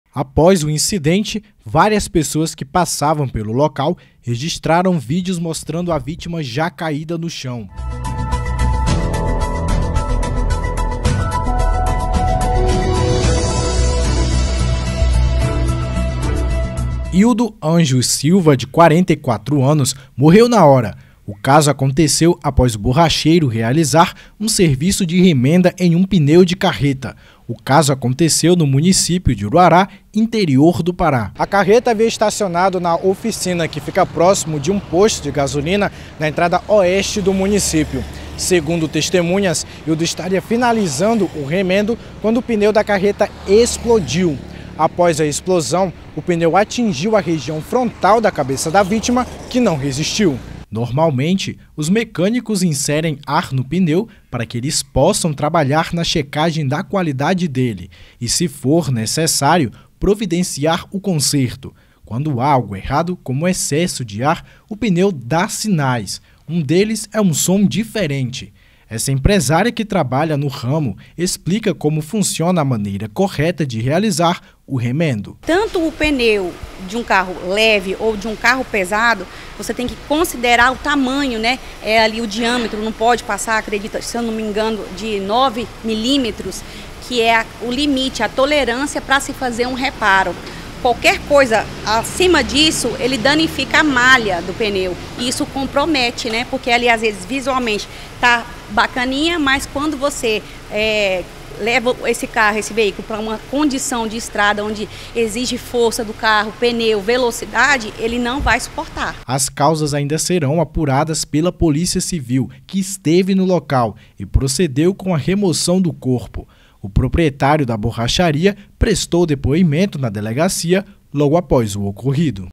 Homem morre após pneu de carreta estourar em Uruará (PA) | SBT Altamira
Nesta quarta-feira (23) um homem morreu enquanto remendava o pneu de um caminhão em Uruará.